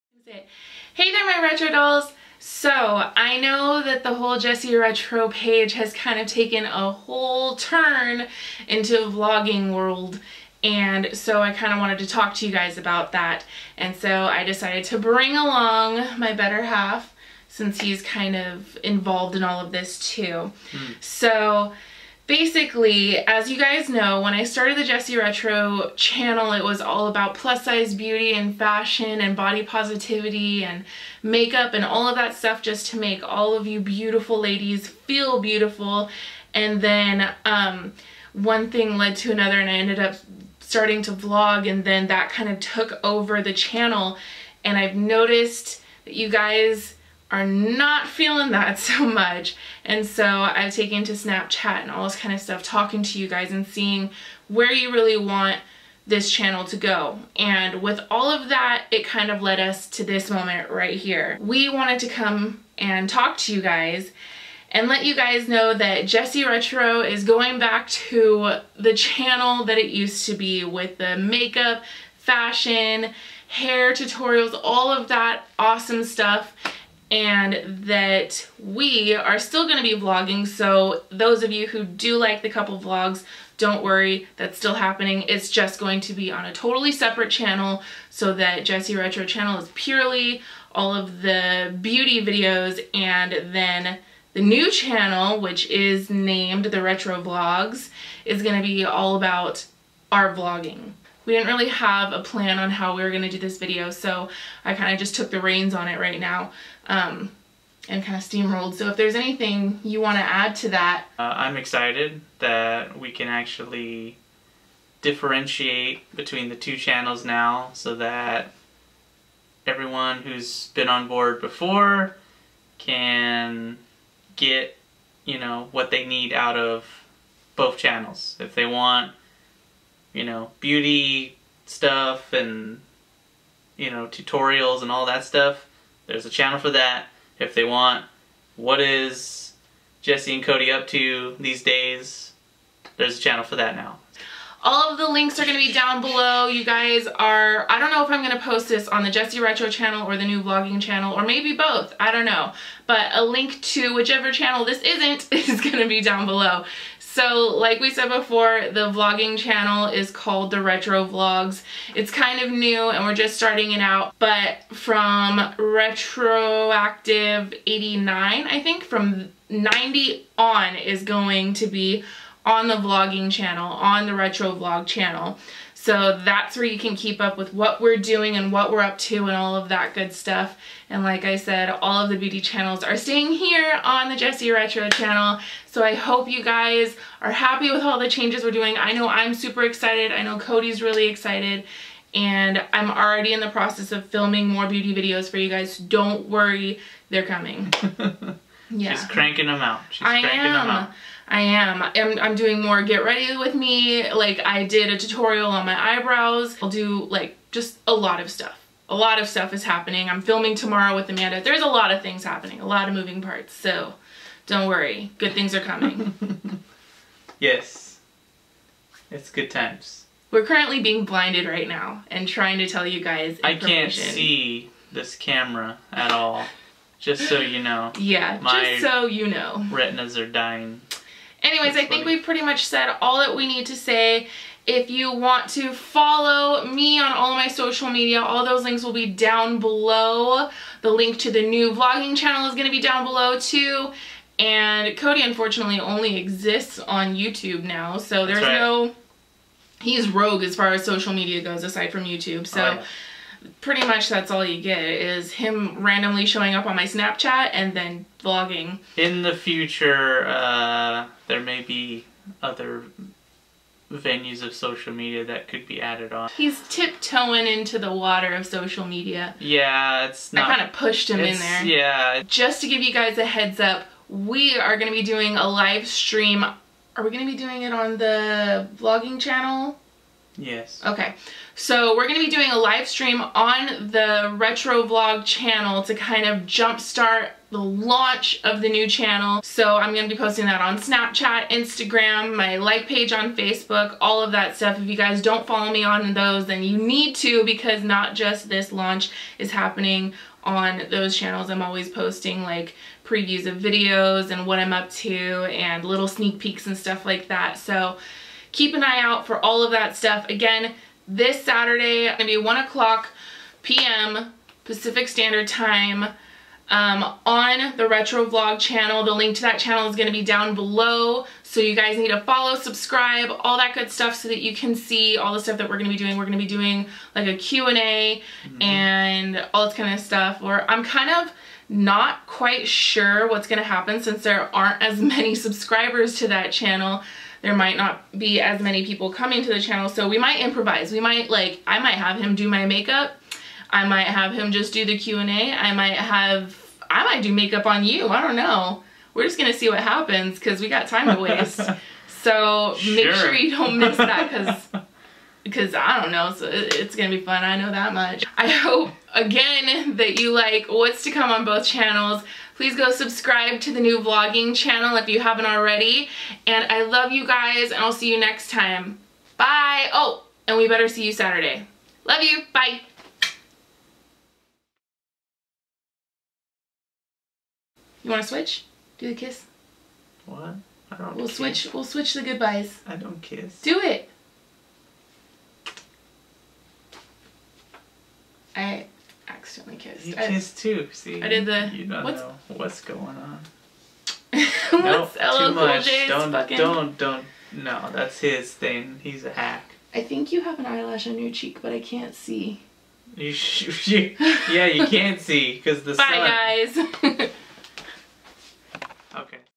EXCITING NEWS!!!
I'm super excited for the future of both channels and I hope you are too!
Please go subscribe to The Retro Vlogs channel and come hang out with us and ask us all the questions you want on the live stream THIS SATURDAY (March 26th @ 1pm PST) on the new vlog channel.

LIVE STREAM: March 26th @ 1pm PST on The Retro Vlogs channel